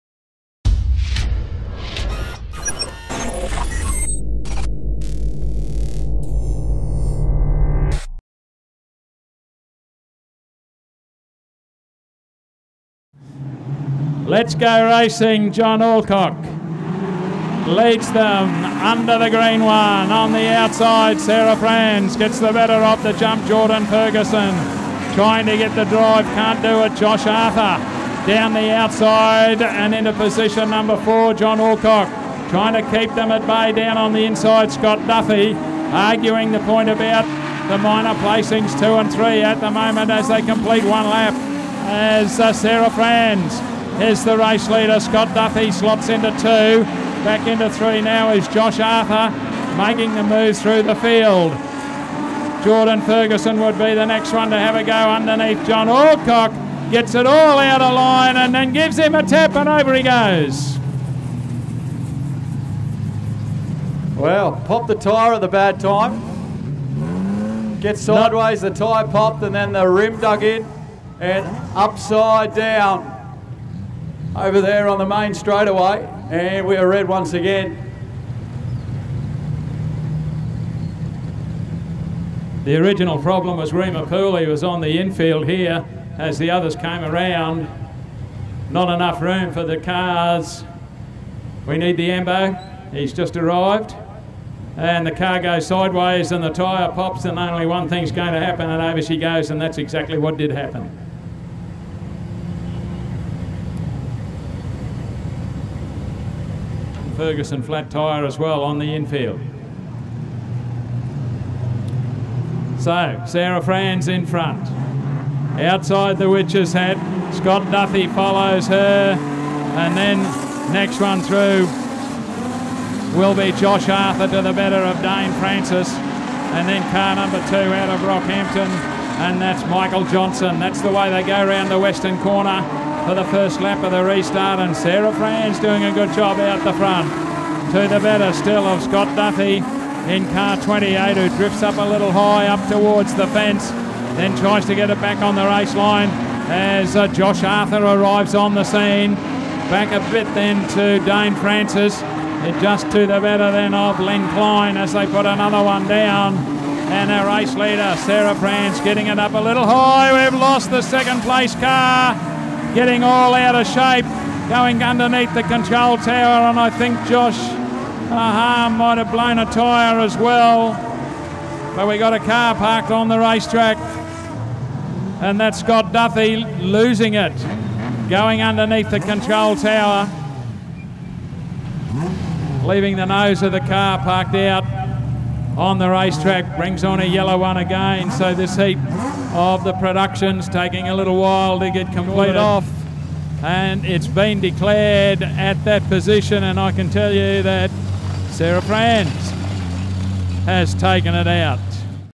Production Sedans - Heat 4 - Kingaroy Speedway - 26.01.17
This is Heat 4 of the Production Sedans held at Lucas Oil Kingaroy Speedway on the 26.01.17